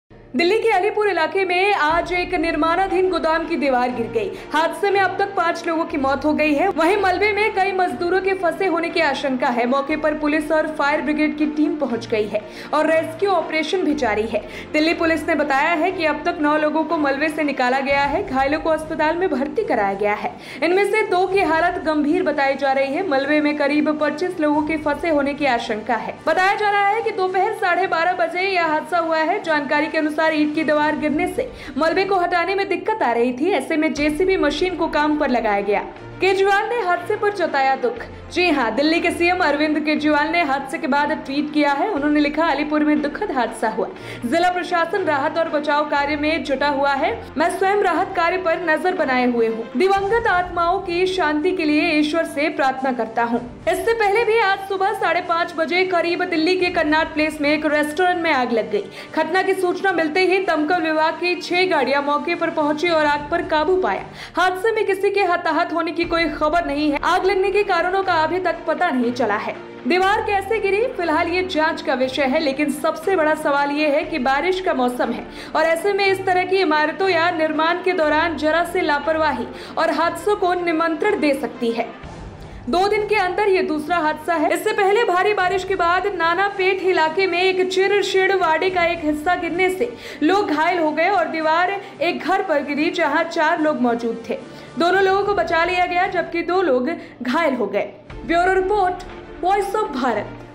Delhi:दिल्ली के अलीपुर इलाके में एक निर्माणाधीन गोदाम की दीवार गिरी
दिल्ली के अलीपुर इलाके में शुक्रवार को एक निर्माणाधीन गोदाम की दीवार गिर गई। हादसे में अब तक 5 लोगों की मौत हो गई। वहीं, मलबे में कई मजदूरों के फंसे होने की आशंका है। मौके पर पुलिस और फायर ब्रिगेड की टीम पहुंच गई है और रेस्क्यू ऑपरेशन जारी है।दिल्ली पुलिस ने बताया कि अब तक 9 लोगों को मलबे से निकाला गया है। घायलों को अस्पताल में भर्ती कराया गया है। इनमें से 2 की हालत गंभीर बताई जा रही है। मलबे में करीब 25 लोगों के फंसे होने की आशंका है।